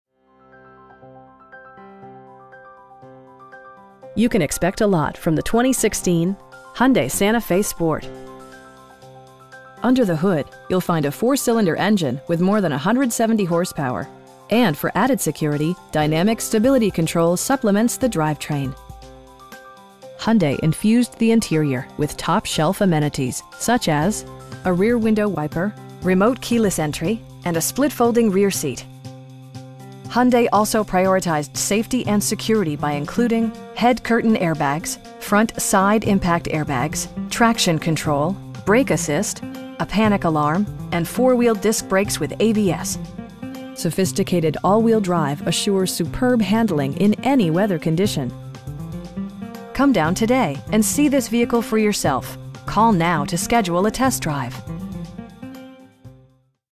2016 Hyundai Santa Fe Sport 4DR AWD 2.4
Conicelli Hyundai
1208 Ridge Pike

You can expect a lot from the 2016 Hyundai Santa Fe Sport. Under the hood you'll find a 4 cylinder engine with more than 170 horsepower, and for added security, dynamic Stability Control supplements the drivetrain. Hyundai infused the interior with top shelf amenities, such as: a rear window wiper, air conditioning, tilt and telescoping steering wheel, remote keyless entry, cruise control, an overhead console, and a split folding rear seat. Hyundai also prioritized safety and security by including: head curtain airbags, front side impact airbags, traction control, brake assist, a panic alarm, and 4 wheel disc brakes with ABS. Sophisticated all wheel drive assures superb handling in any weather condition. Come down today and see this vehicle for yourself.